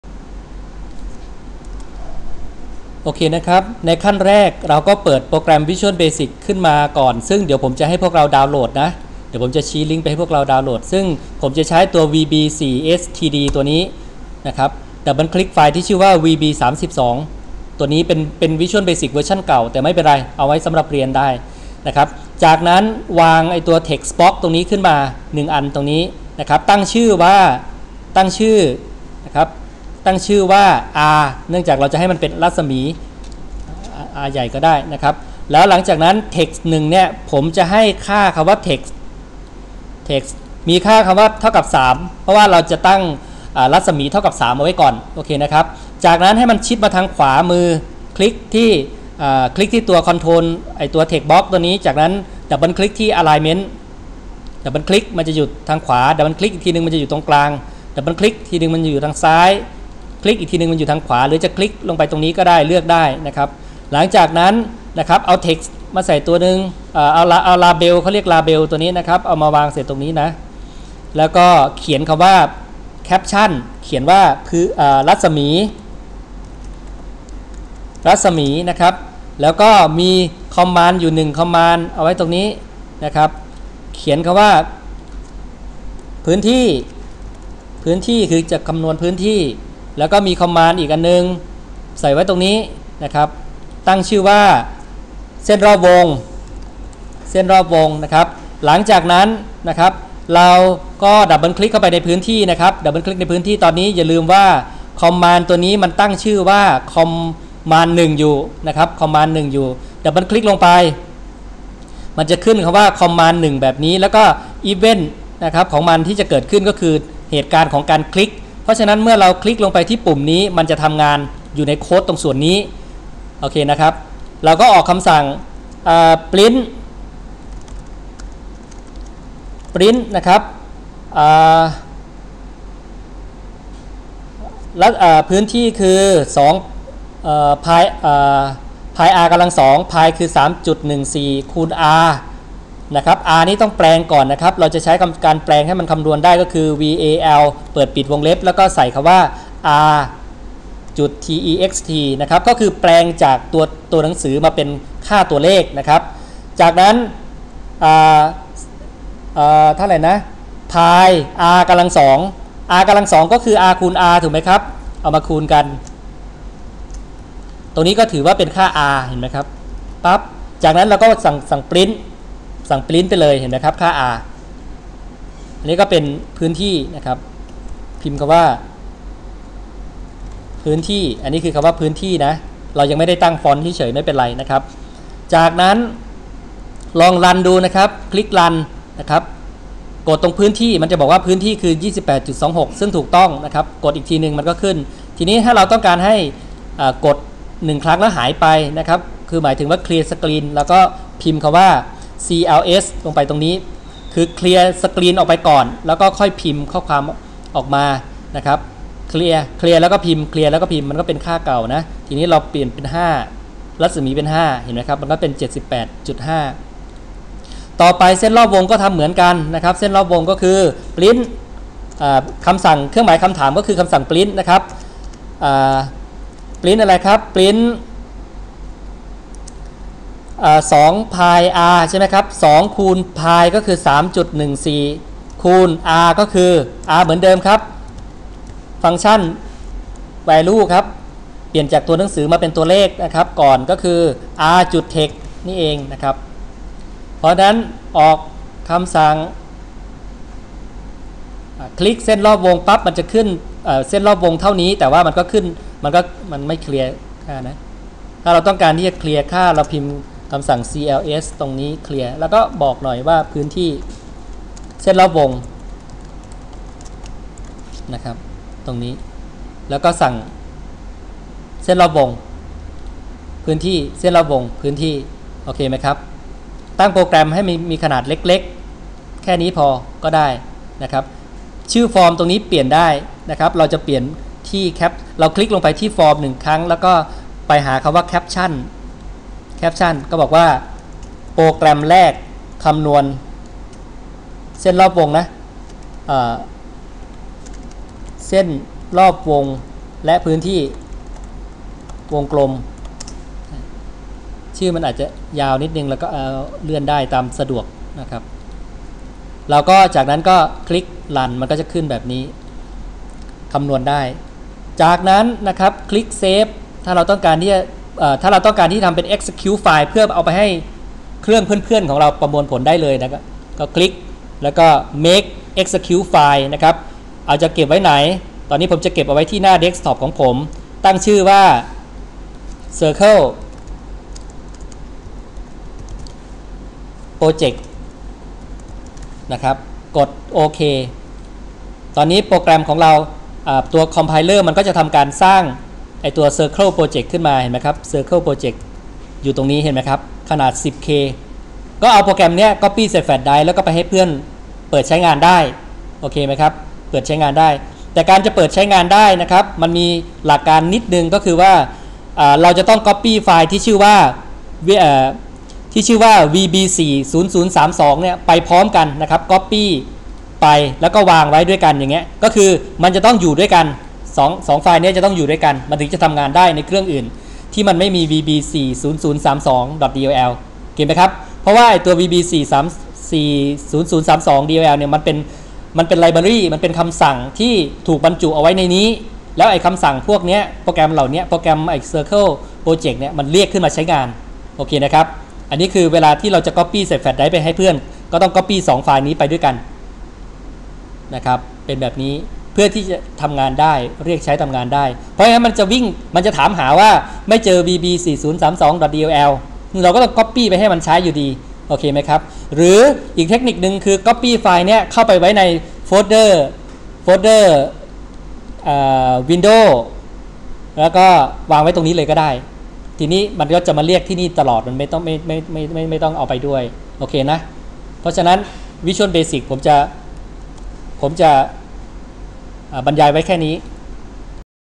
visual basic sample [thai]
อธิบาย ตัวอย่างการเขียนโปรแกรมคำนวณพื้นที่วงกลมและเส้นรอบวงกลมด้วย วิชวลเบสิก พอเป็นสังเขป